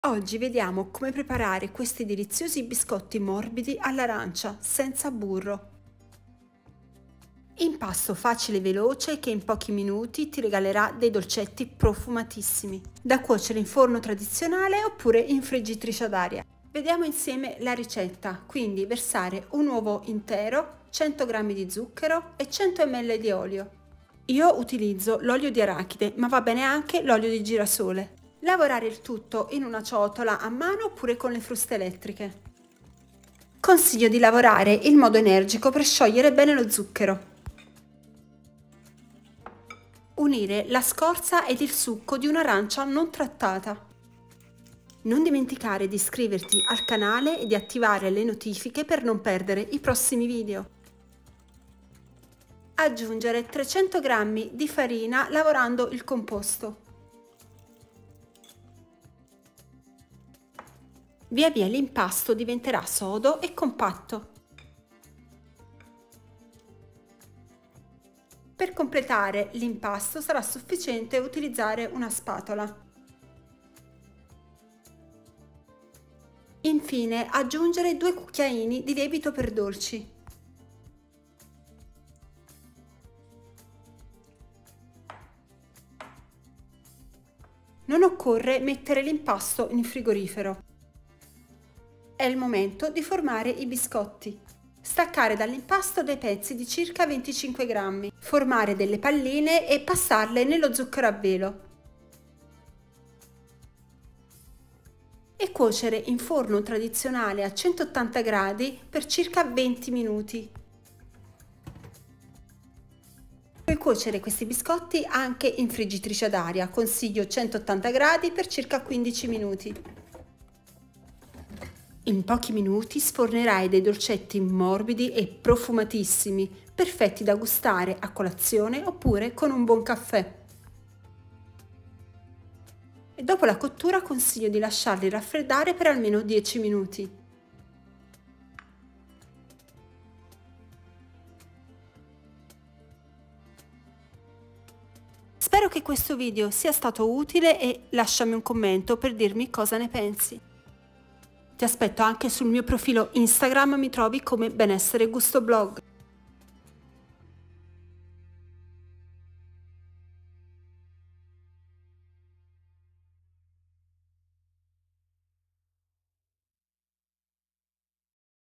Biscotti facilissimi e morbidi all’arancia senza burro
Biscotti morbidissimi e profumati con scorza e succo d'arancia.
Ingredienti:

100 ml Olio di semi di arachide
100 g Zucchero
1 Uovo medio
Scorza di mezza arancia
succo di mezza arancia
300 g farina 00
8 g Lievito in polvere per dolci ((mezza bustina))
Cottura in forno statico 180 gradi per 15/20 minuti oppure friggitrice ad aria 180 gradi per 15 minuti